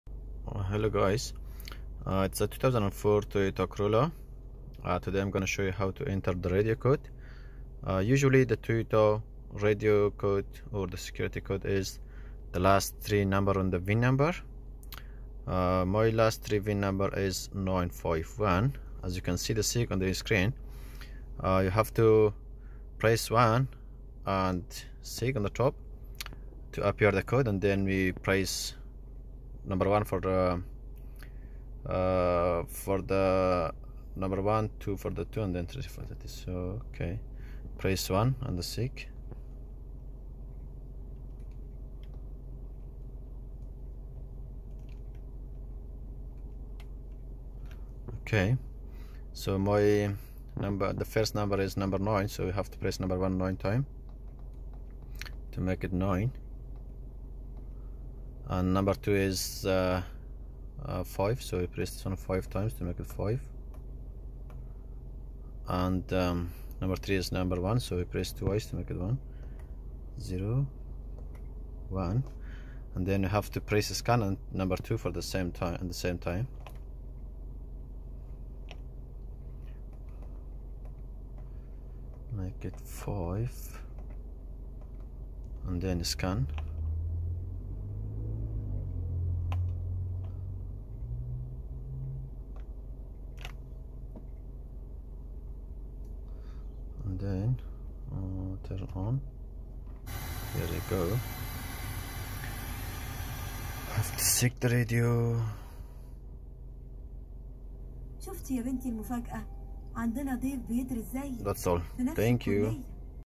2003 to 2008 toyota corolla how to inter radio code
Usually toyota   radio code is the 3 last vin number .